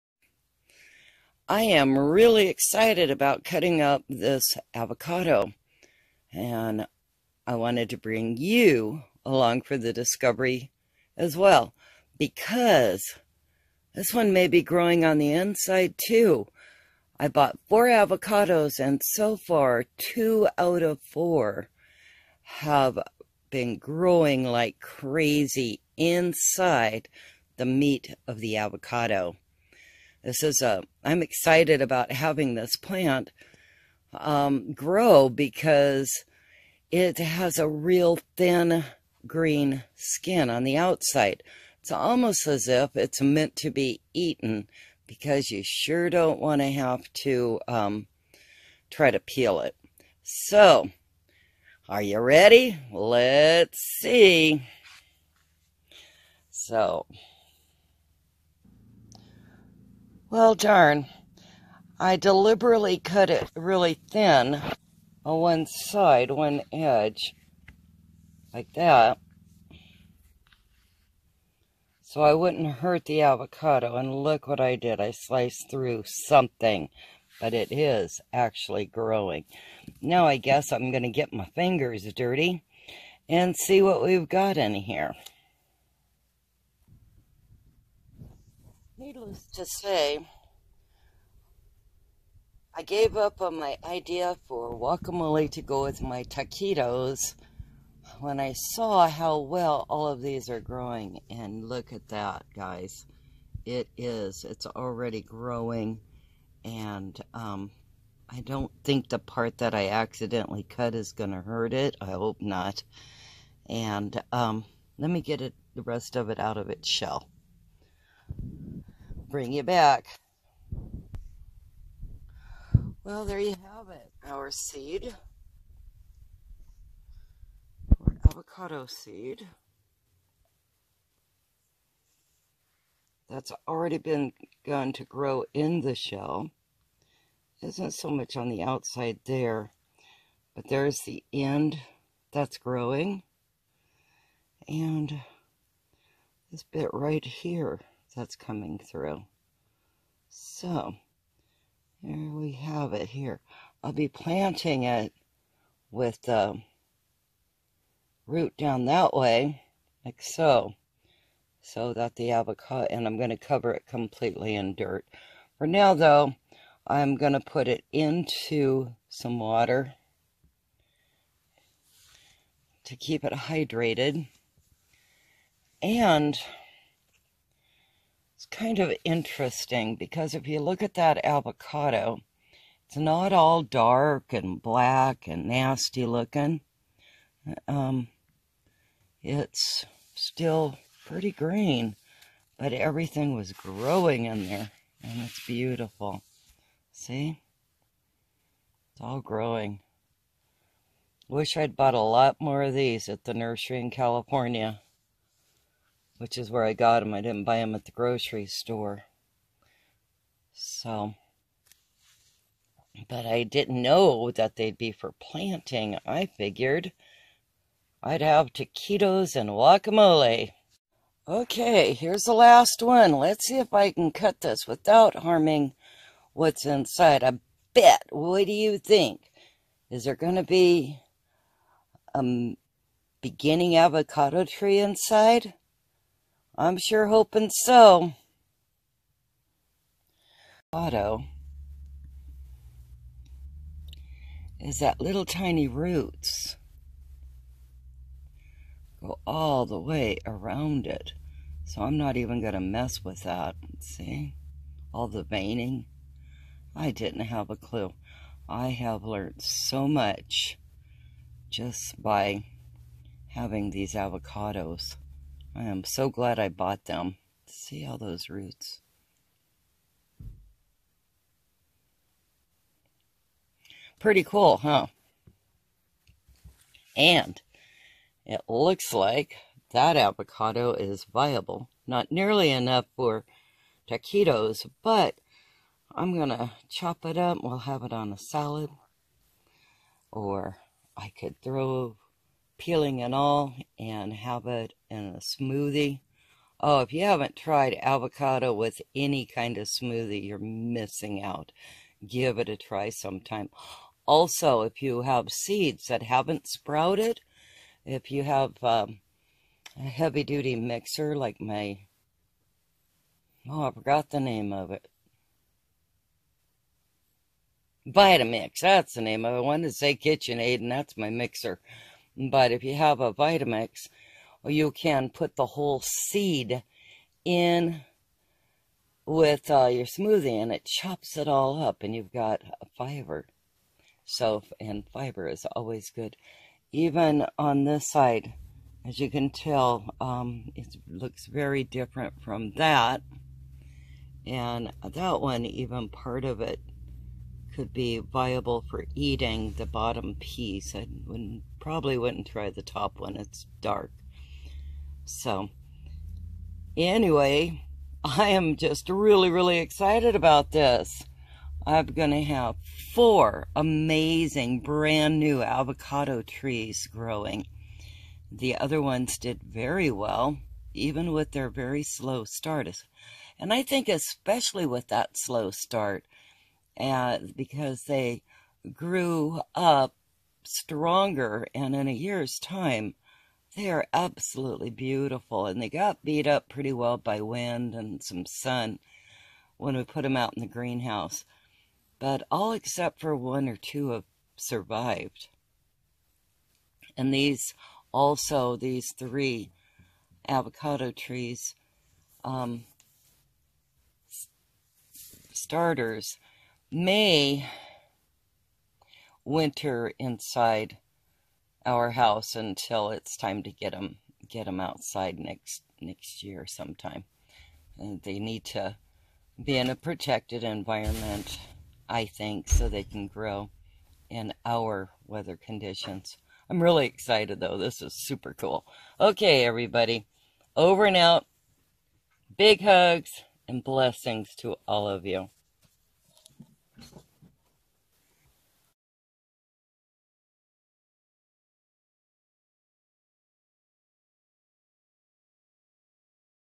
Organic Avocado 🥑 Sprouting Inside Fruit
I like to plant avacodo seeds. I know the fruit will not be the same as the mother tree and it will take forever like at least 10 years to get a harvest. But...I keep growing them! Eventually we will air layer the trees with an avocado 🥑 tree that grows well in our hot summers. Trees make great shade and wind block.